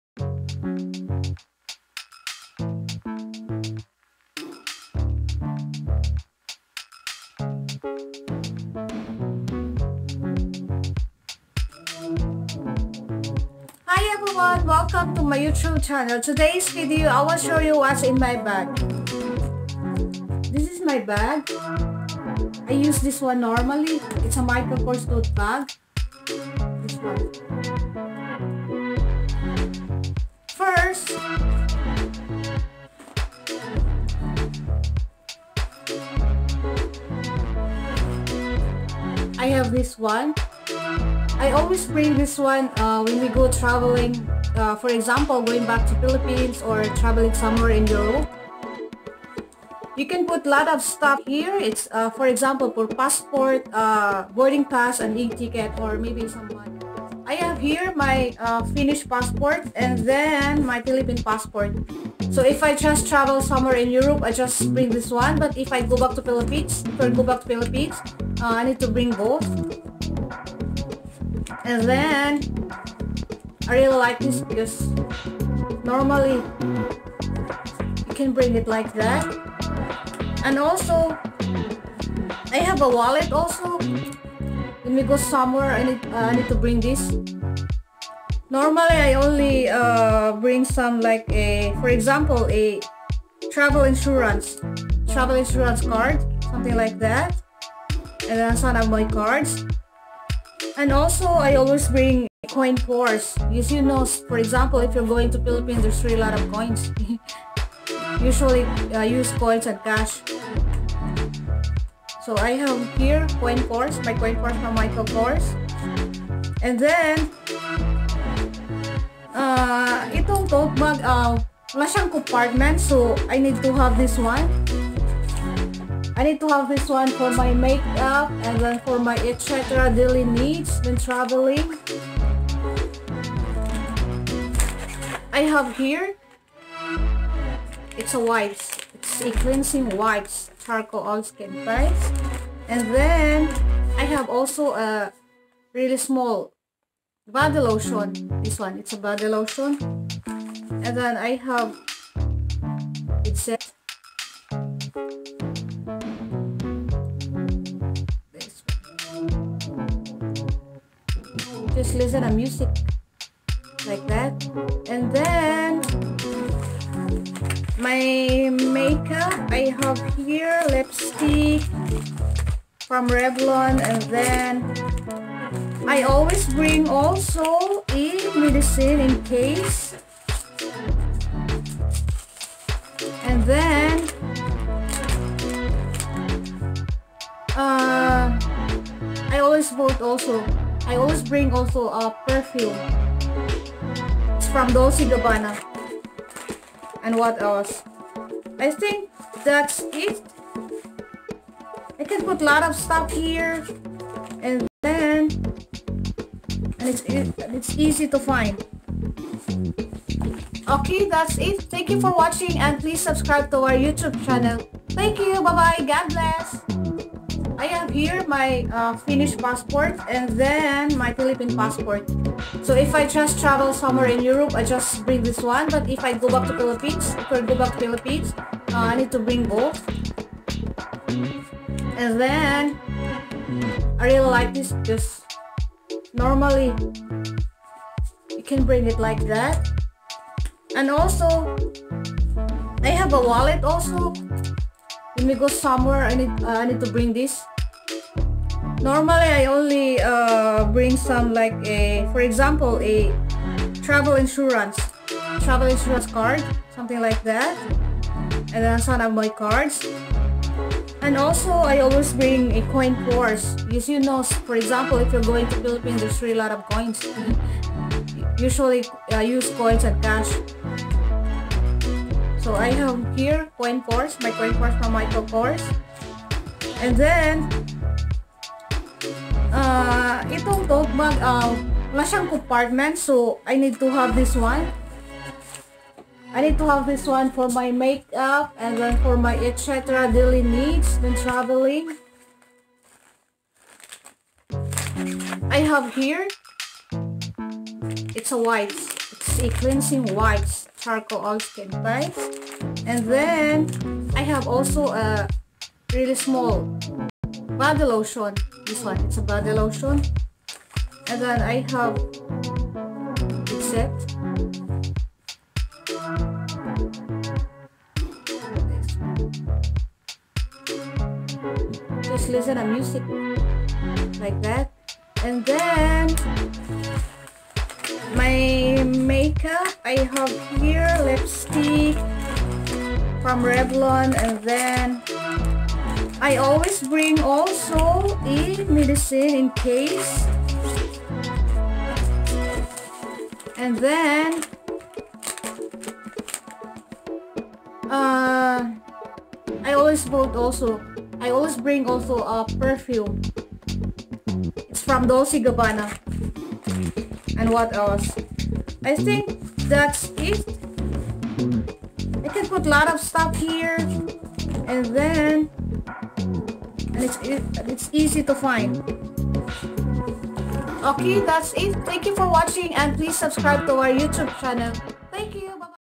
What's in My Bag| Michael Kors Tote Bag
Hi everyone I will show you what's in my bag and what are the things I need to bring when travelling.